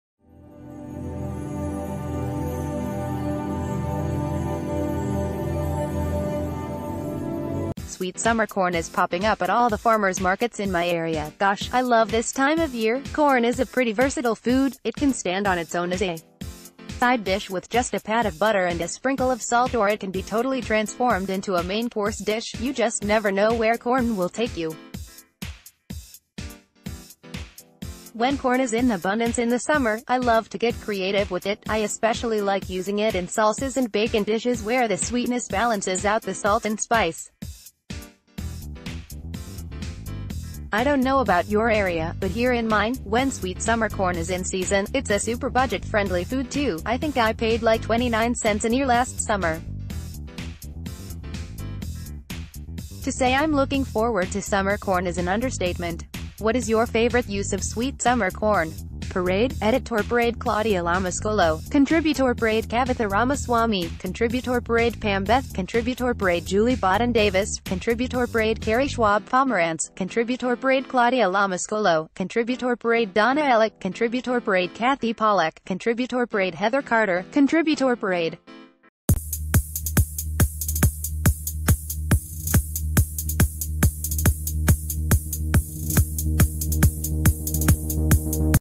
Breaking News | 11 Super Sweet Summer Corn Recipes
Sweet summer corn is popping up at all the farmer’s markets in my area.   Gosh, I love this time of  year!Corn is a pretty versatile food.  It can stand on its own as a side dish with just a pat of butter and a sprinkle of salt or it can be totally transformed into a main course dish.  You just never know where corn will take you. When corn is in abundance in the summer, I love to get creative with it.  I especially like using it in salsas and bacon dishes where the sweetness balances out the sa...